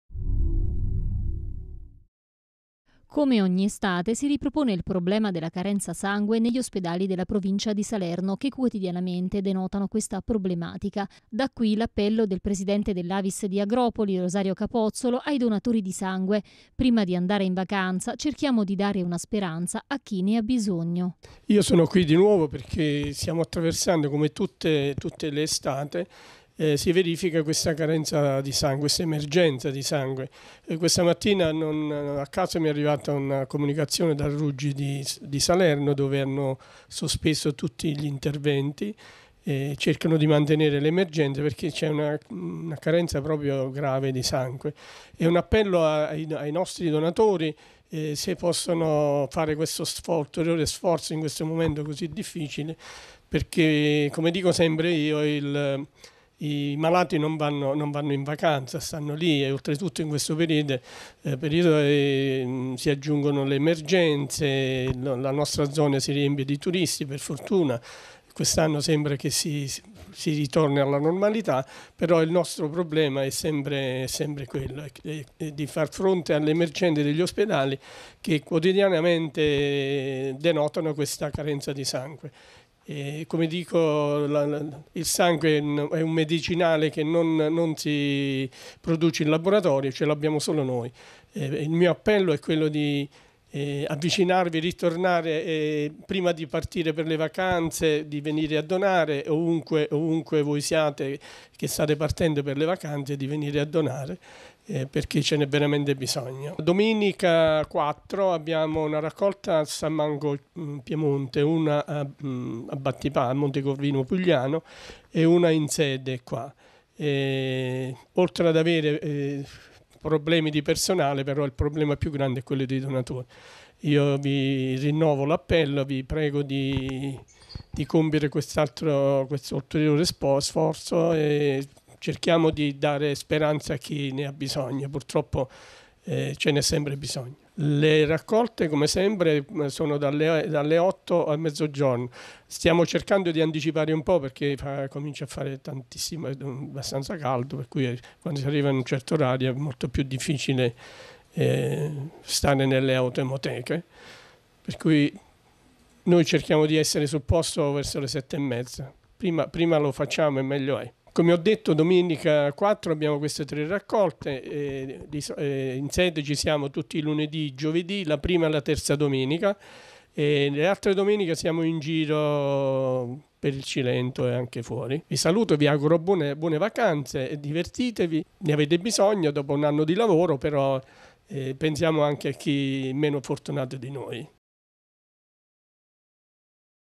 Agropoli, Carenza di sangue nella provincia di Salerno, l’appello del pres. Capozzolo ai donatori
Come ogni estate si ripropone il problema della carenza sangue negli ospedali che quotidianamente denotano questa problematica.L’ appello del presidente dell’Avis di Agropoli, Rosario Capozzolo ai donatori di sangue: “Prima di andare in vacanza cerchiamo di dare speranza a chi ne ha bisogno”.